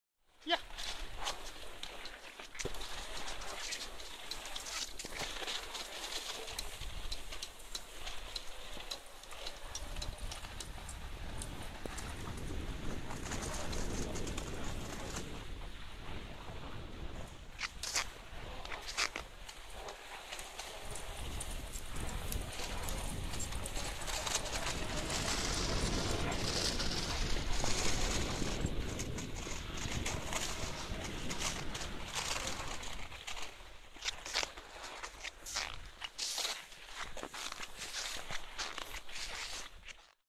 Finnmarksberner/Idita-BMD?
Det er ikke bare huskyer som kan trekke. Bernere gjør også nytta.
Pulling on snow is not huskies only. BMDs are also able.